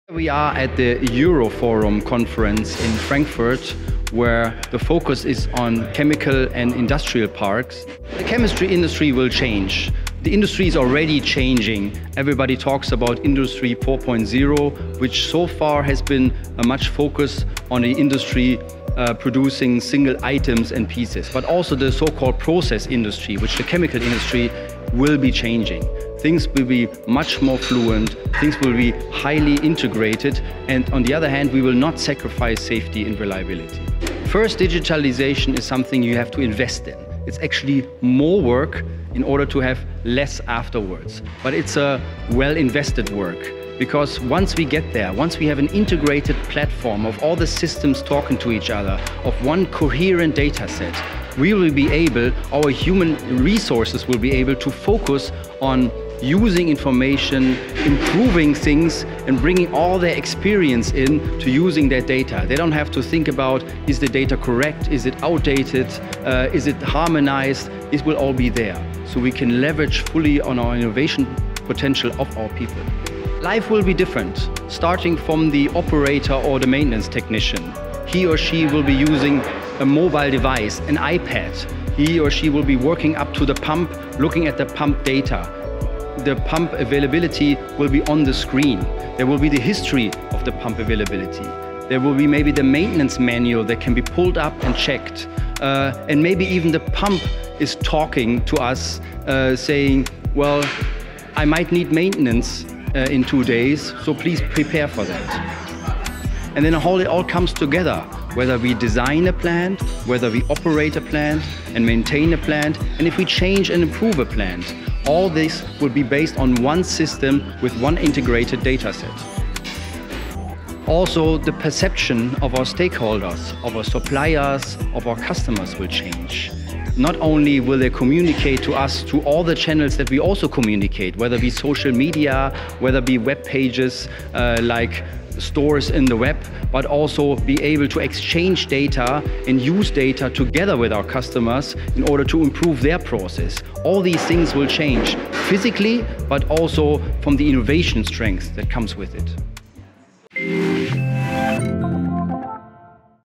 Dr. Hanno Brümmer on digitalization of the chemical industry
Dr. Hanno Brümmer, Head of Production and Technology at Covestro Polyurethanes, talking about digitalization of the chemical industry at Handelsblatt Euroforum Chemistry & Industrial Parks, Frankfurt 6 December 2018.